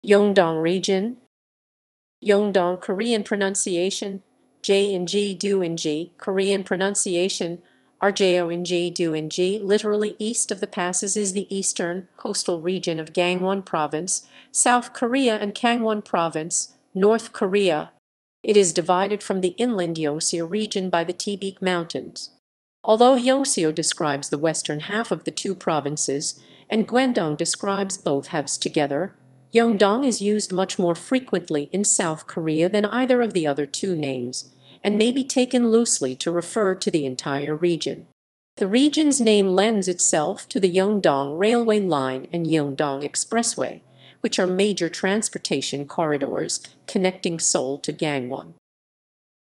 Yeongdong (region)
Yeongdong (Korean pronunciation: [jʌŋ.doŋ], Korean pronunciation: [ɾjɔŋ.dɤŋ], literally 'east of the passes') is the eastern, coastal region of Gangwon Province, South Korea and Kangwŏn Province, North Korea. It is divided from the inland Yeongseo region by the Taebaek Mountains.
Although Yeongseo describes the western half of the two provinces and Gwandong describes both halves together, "Yeongdong" is used much more frequently in South Korea than either of the other two names, and may be taken loosely to refer to the entire region.
The region's name lends itself to the Yeongdong Railway Line and Yeongdong Expressway, which are major transportation corridors connecting Seoul to Gangwon.